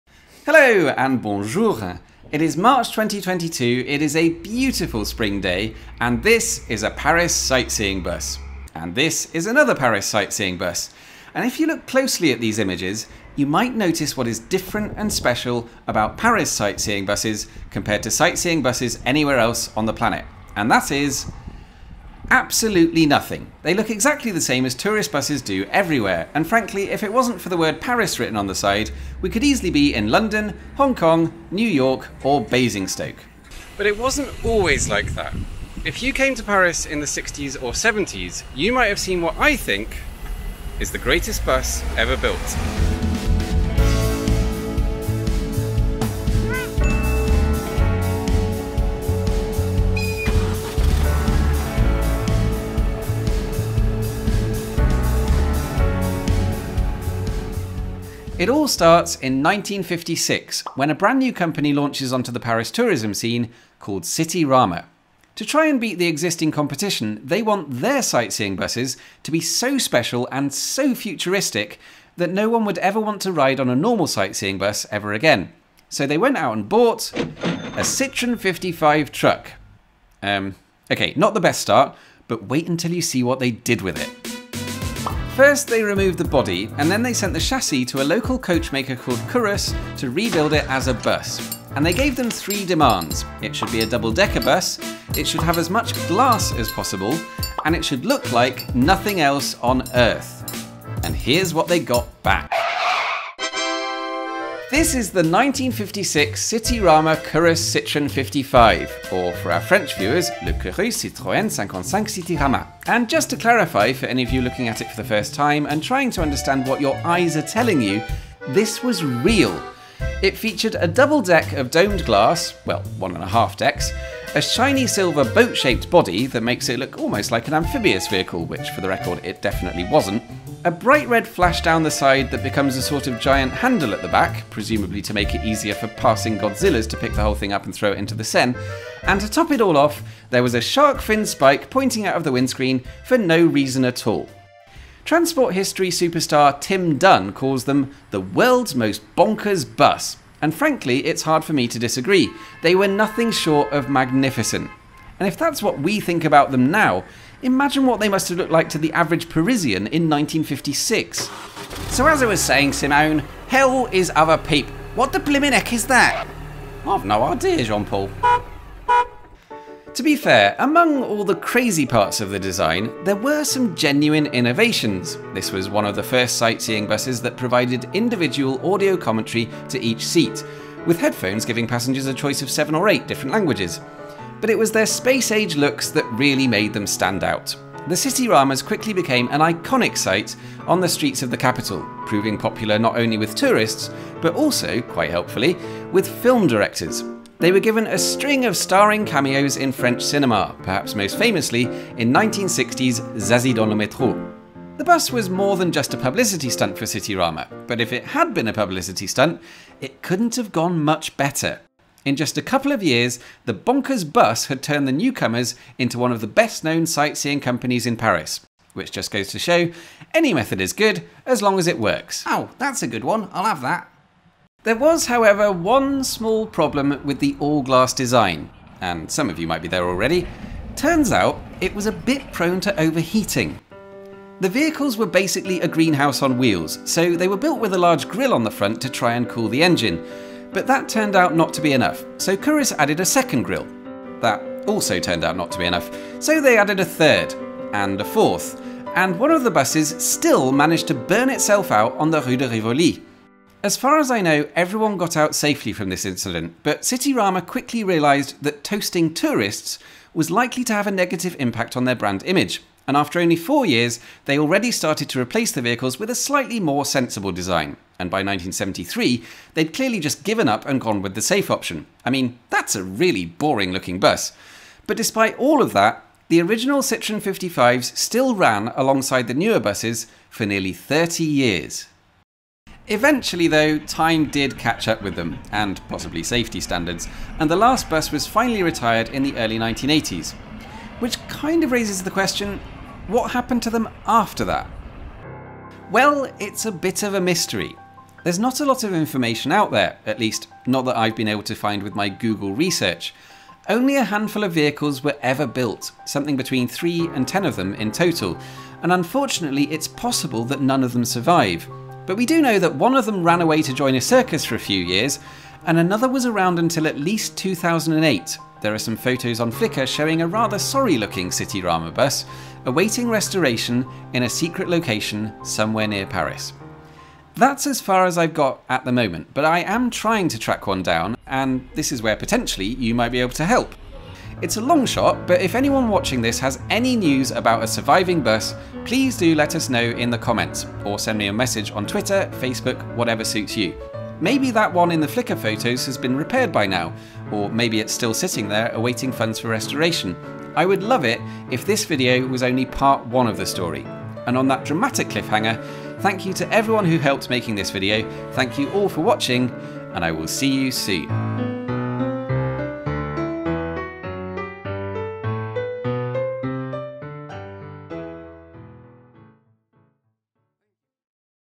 The Cityrama Citroen 55: The Strangest Bus Ever Built?
In 1956, a company called Cityrama decided to shake up the Paris sightseeing industry, by building the world's most outrageously ridiculous bus. Was it utterly brilliant? Yes. Did it have any catastrophic design flaws? Also yes...

IMAGE CREDITS